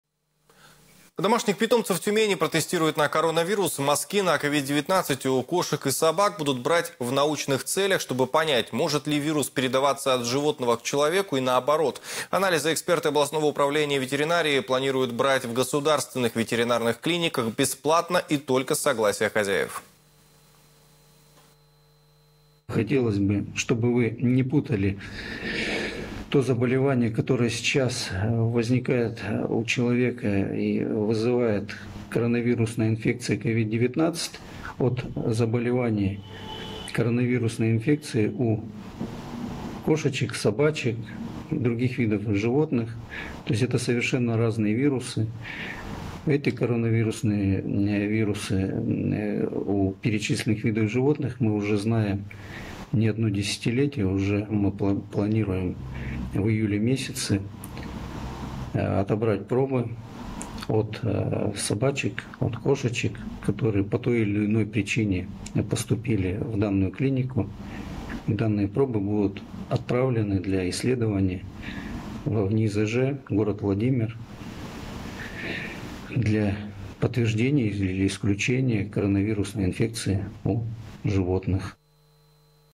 Коронавирус у животных
Домашних питомцев в Тюмени протестируют на коронавирус. Мазки на COVID-19 у кошек и собак будут брать в научных целях, чтобы понять, может ли вирус передаваться от животного к человеку и наоборот. Анализы эксперты областного управления ветеринарии планируют брать в государственных ветеринарных клиниках бесплатно и только с согласия хозяев.